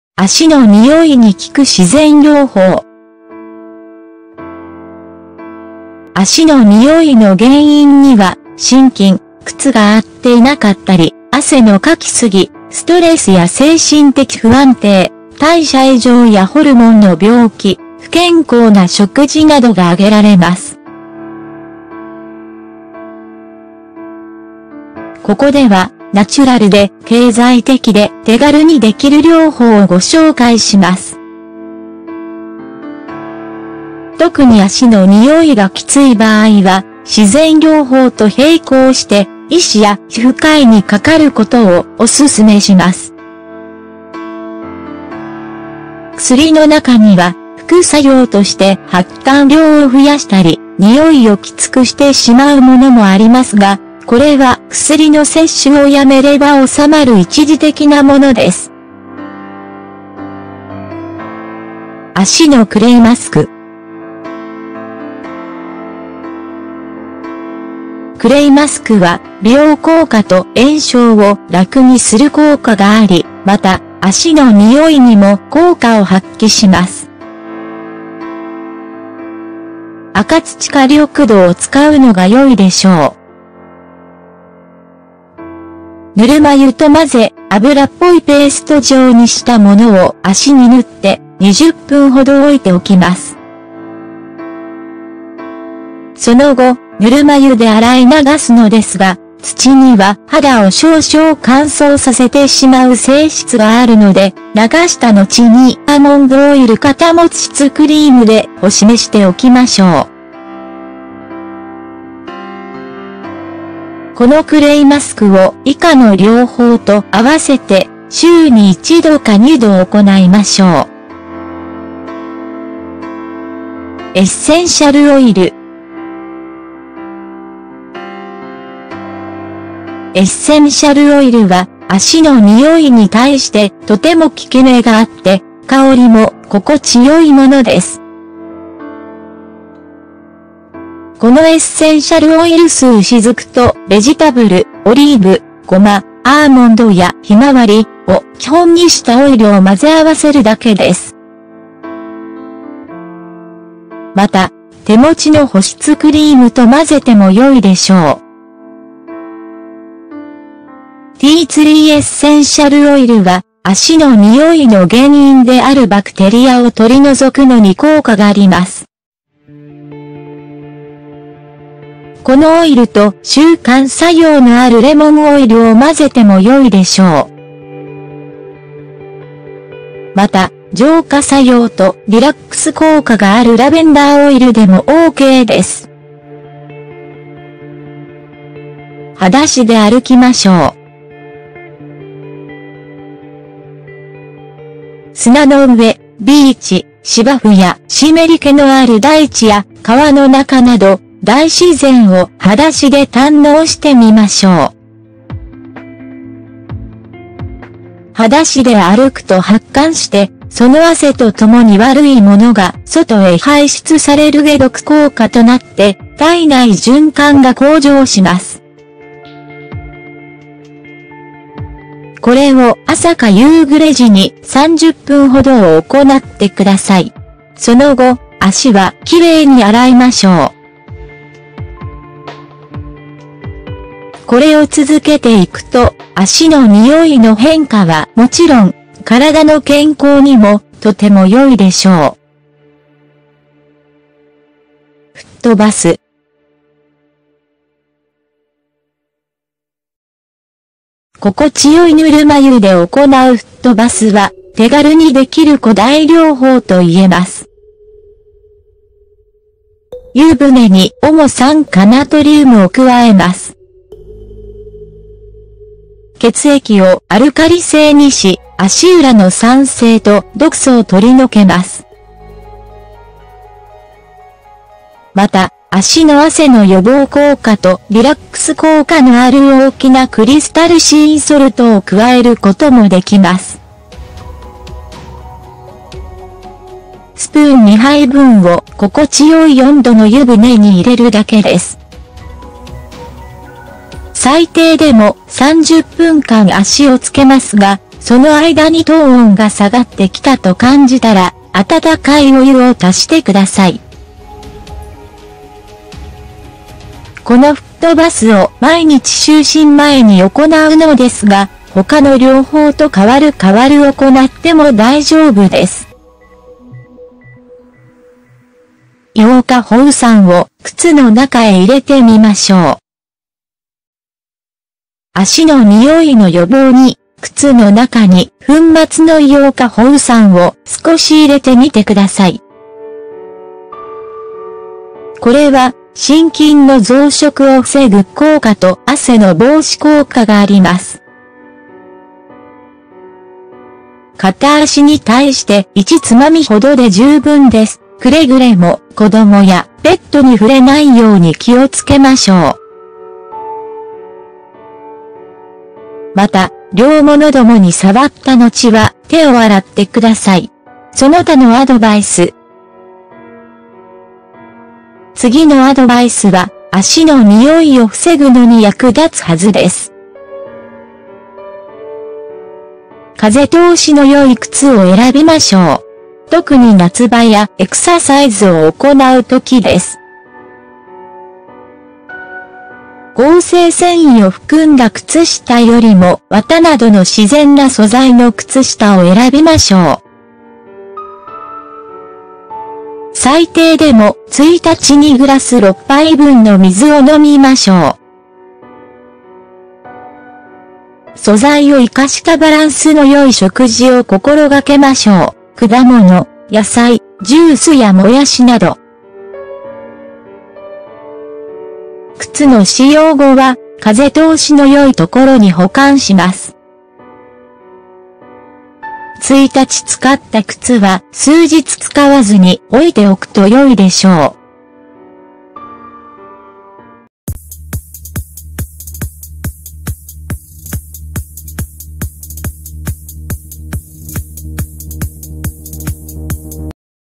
足の匂いに効く自然療法
足の匂いの原因には、真菌、靴が合っていなかったり、汗のかき過ぎ、ストレスや精神的不安定、代謝異常やホルモンの病気、不健康な食事などが挙げられます。ここでは、ナチュラルで経済的で手軽にできる療法をご紹介します。